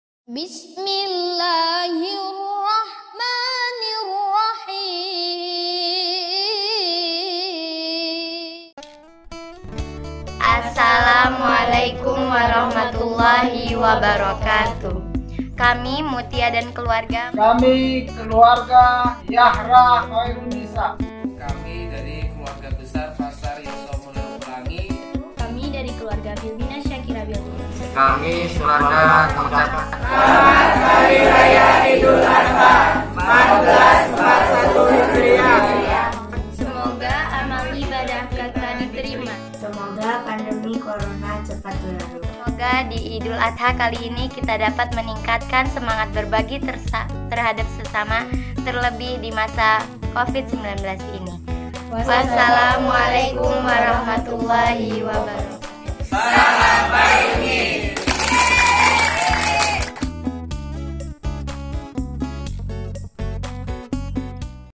Tahniah Idhul Adha 1441 H SMP Muhammadiyah Ahmad Dahlan Metro
Kolaborasi antara wali dan siswa/i dalam kreasi Tahniah Idhul Adha 1441 H.